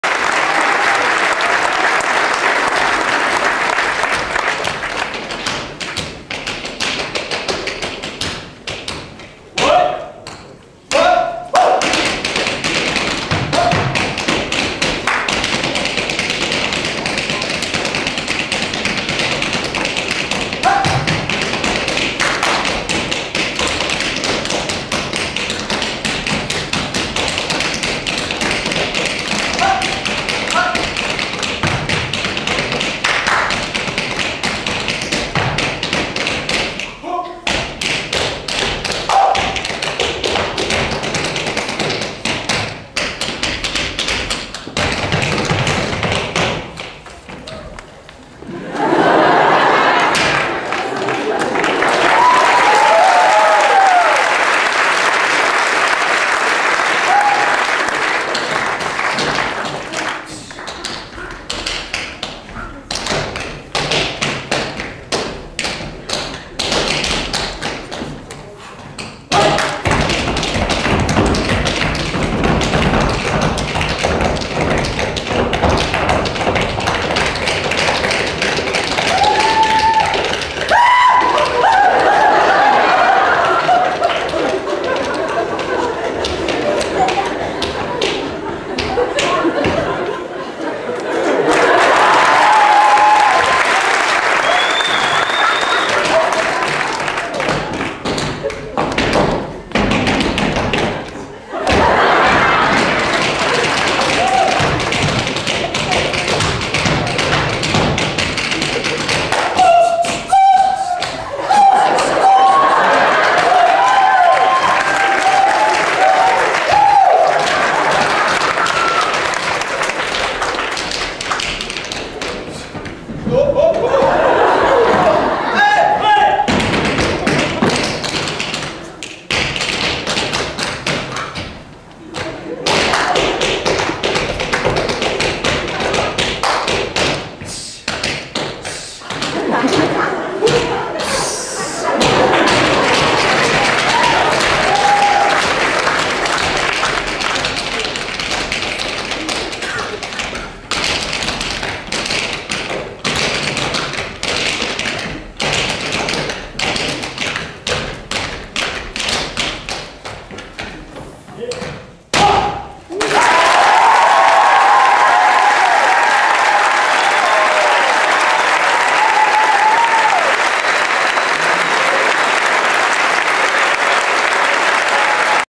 MSF2010j_CaD.avi
Costel a Dorel Surbeck na Mezinárodním stepařském festivalu Brno 2010